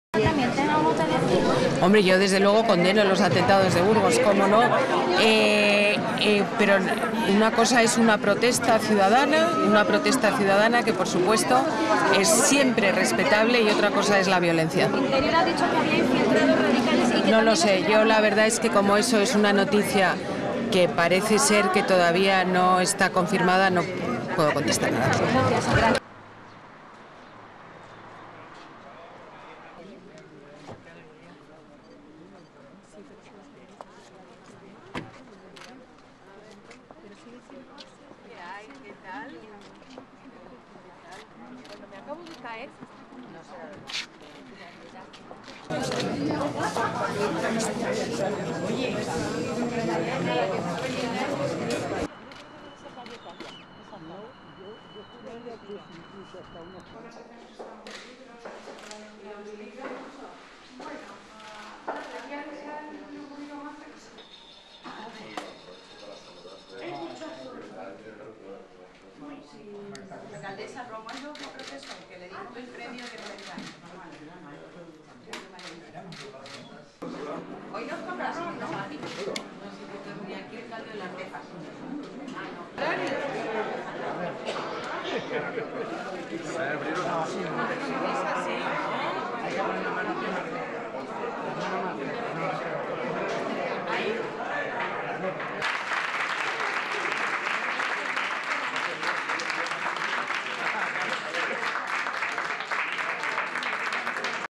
Ana Botella habla de los 'atentados' en Burgos
La alcaldesa de Madrid, Ana Botella, ha mostrado este lunes su rechazo a los disturbios ocurridos durante las protestas ciudadanas que se llevan produciendo desde el pasado viernes en el barrio de Gamonal (Burgos) por las obras de remodelación de la calle Vitoria.